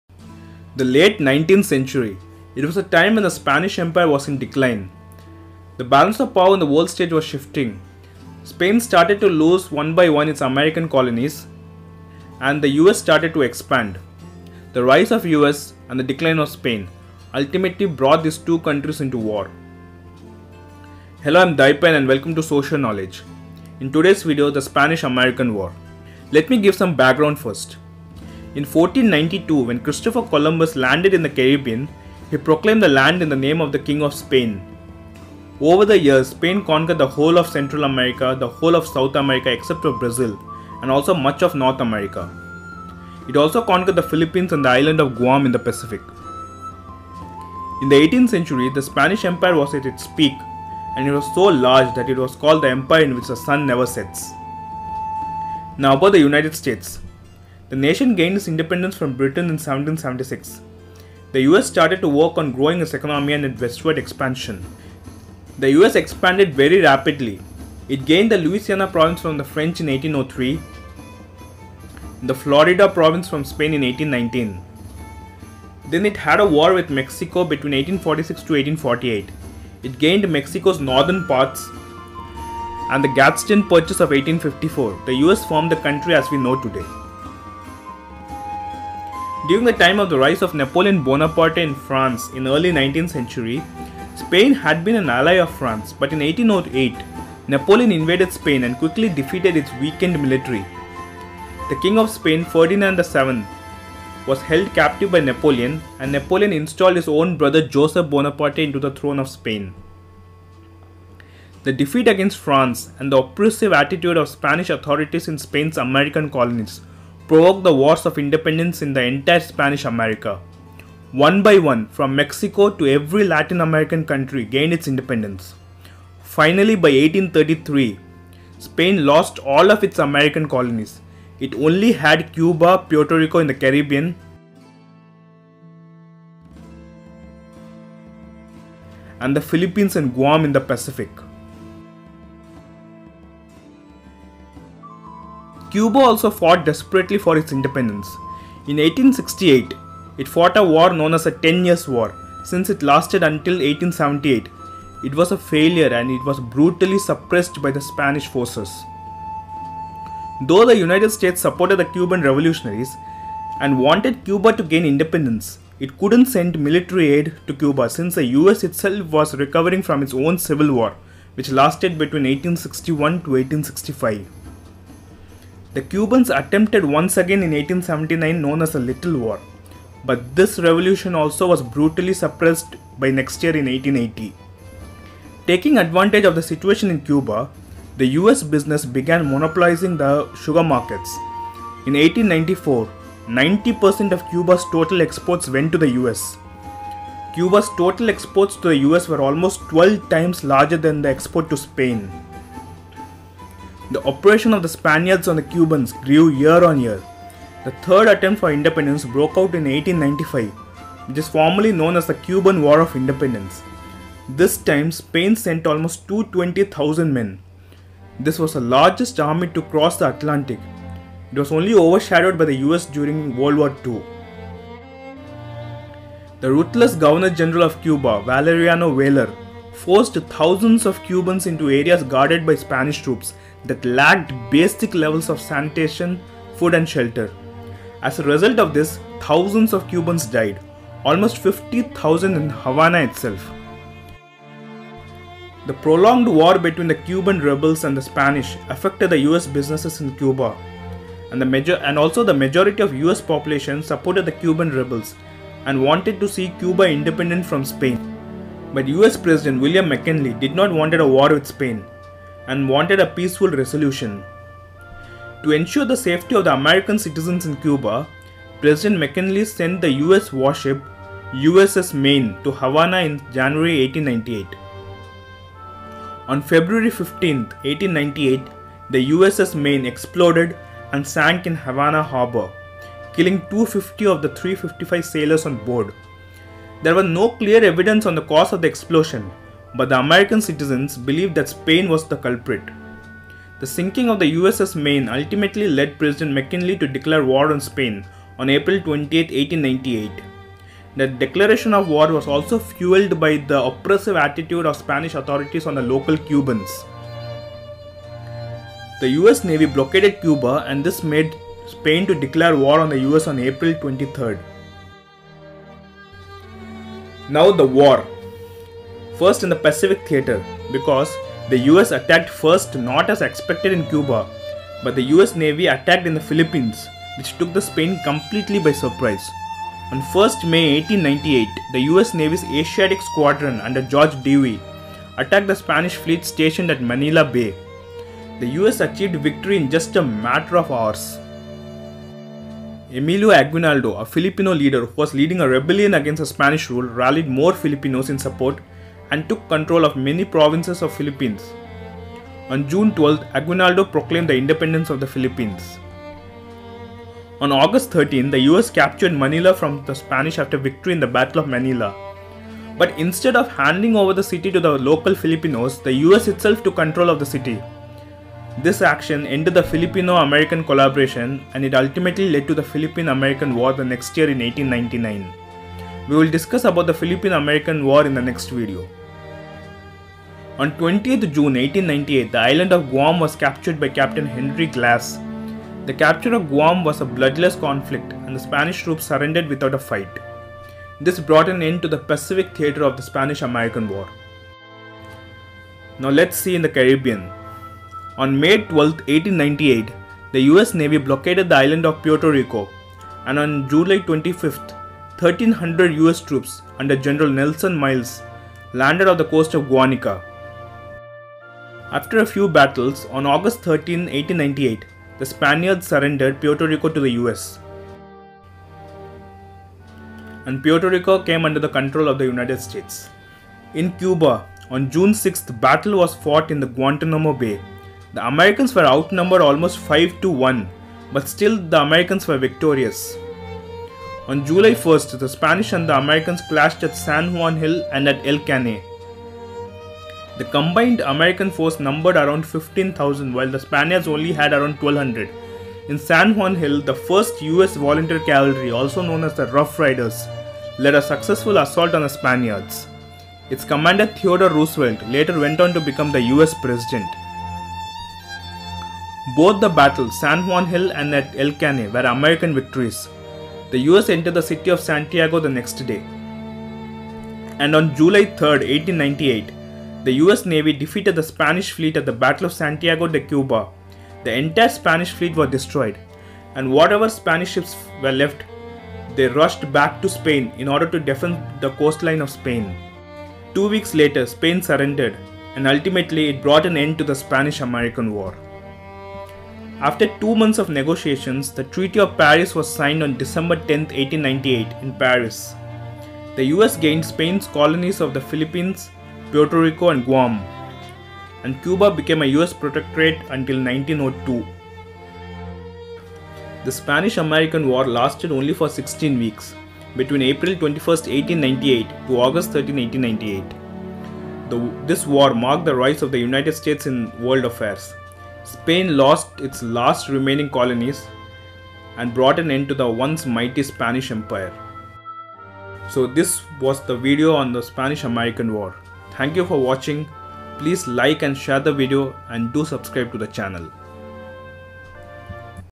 The Spanish-American War Explained in 9 Minutes | Why did the Spanish American War happen
The Spanish-American War Explained in 9 Minutes
Why did the Spanish American War happen | What are the causes and effects of the Spanish American War? | spanish american conflict over cuba

Late 19th century, the power of the once-mighty Spanish Empire was in decline. The balance of power in the world was shifting & it was the rise of U.S expansionism.

In 1492, when Christopher Columbus landed on the Caribbean shores, he proclaimed the land in the name of the King of Spain. Over the years, Spain conquered the whole of Central America, the whole of South America except for Brazil & half of North America. In the Pacific, it also conquered the Philippines & the island of Guam.

The US gained its independence from Britain in 1776. During the time of the rise of Napoleon Bonaparte in France in the early 19th century, Spain had been an ally of France. But in 1808, Napoleon invaded Spain and quickly defeated its weakened military. The defeat against France & the oppressive attitude of Spanish authorities in Spain’s American colonies provoked the wars of independence in entire Spanish America. One by one from Mexico to every Latin American country gained its independence from Spain by 1833.
Finally all but Spain had was Cuba, Puerto Rico in the Caribbean & the Philippines & Guam in the Pacific.

Cuba fought desperately for its independence in 1868. This war is known as the “Ten Years War” since it lasted until 1878 was a failure & was suppressed by the Spanish forces.
Though the United States supported the Cuban revolutionaries and wanted Cuba to gain independence, it couldn’t send military aid to Cuba since the US itself was recovering from its own Civil War which lasted between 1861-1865. The Cubans attempted once again in 1879 known as the “Little War” which was too was brutally suppressed by 1880.

The third attempt for independence broke out in 1895 as formally known as the “Cuban War of Independence”.
This time Spain sent almost 220 thousand men. This was the largest army ever to cross the Atlantic only to be overshadowed by the U.S during WW II.
The ruthless Governor-General of Cuba, Valeriano Weylar forced thousands of Cubans into areas guarded by Spanish troops that lacked a basic level of sanitation, food & shelter. As a result of this, thousands died. Almost 50 thousand in Havana itself.
To ensure the safety of the American citizens in Cuba, President McKinley sent the U.S warship USS Maine to Havana in January 1898.
On February 15th, 1898, the USS Maine exploded and sank in Havana harbor killing 250 of the 355 sailors on board. The sinking of the USS Maine ultimately led President McKinley to declare war on Spain on April 20th, 1898. The US Navy blockaded Cuba & this made Spain declare war on the U.S on April 23rd, 1898.

On 1st May’1898, the U.S Navy’s Asiatic Squadron under George Dewey attacked the Spanish fleet stationed at Manila Bay. The U.S achieved victory in just a matter of hours.
Emilio Aguinaldo, a Filipino leader who was leading a rebellion against the Spanish rule rallied more Filipinos in support & took control of many provinces of the Philippines. On June 12th, Aguinaldo proclaimed the independence of the Philippines.
On August 13th, the U.S captured Manila from the Spanish after victory in the battle of Manila. Instead of handing over the city to the Filipinos, the U.S itself took control of the city.
The Spanish General, Fermin Jaudenes surrendered to the U.S on Aug 14th, 1898.

In Cuba, on June 6th, battle was fought in Guantanamo Bay. The Americans were outnumbered almost 5 to 1. But still, the Americans were victorious.
Both the battle, San Juan and El Caney were American victories. The U.S entered the city of Santiago the next day.
The U.S Navy defeated the Spanish fleet at the battle of Santiago de Cuba on July 3rd. The entire Spanish fleet was destroyed. Two weeks later, Spain surrendered & subsequently ended the Spanish-American War.
After two months of negotiations, the Treaty of Paris was signed on December 10th, 1898 Paris. The U.S gained Spain’s colonies of the Philippines, Puerto Rico & Guam and Cuba became a U.S protectorate until 1902.
The Spanish-American War lasted for 16 weeks between April 21st, 1898 to Aug 13th, 1898. The War marked the rise of the United States in world affairs. Spain lost the last remnants of its colonial empire & brought an end to the once-mighty Spanish Empire.